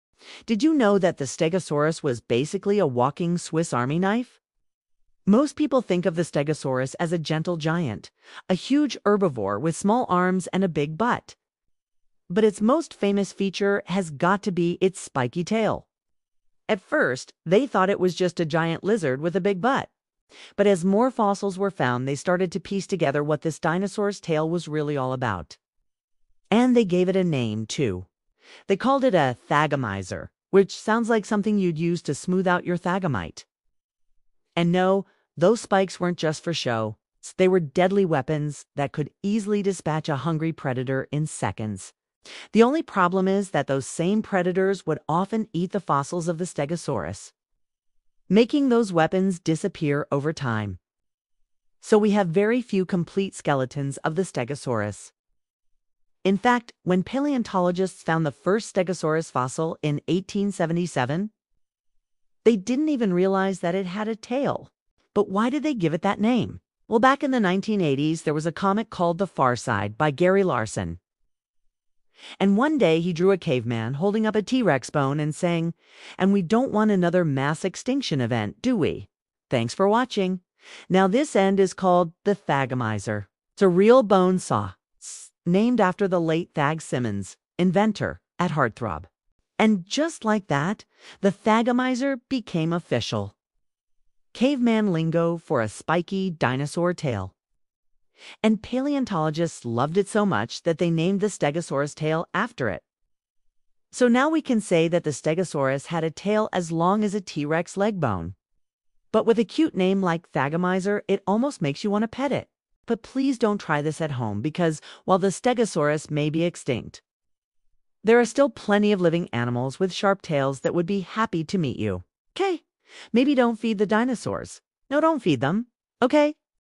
Thagomizer How a COMIC Named the Stegosaurus' Deadly Tail!
🔍 **Why did scientists MISS the Stegosaurus' deadliest feature for 100 years?** Uncover the wild story behind paleontology's most hilarious naming fail:

⚔️ **THAGOMIZER REVEALED**: How Gary Larson's "Far Side" comic named the Stegosaurus' lethal tail spikes
🦴 **Fossil Detective Work**: Why early paleontologists thought it was "just a big butt" (not a weapon!)
💀 **Predator Nightmare**: How these spikes could "dispatch a T-Rex in seconds" (when fossils survived)
🤣 **Science Meets Comedy**: The caveman joke that became OFFICIAL paleontology terminology

**Warning**: Don't try petting modern thagomizers (porcupines/horned lizards agree!)

**Like** if you'll never look at Stegosaurus the same way!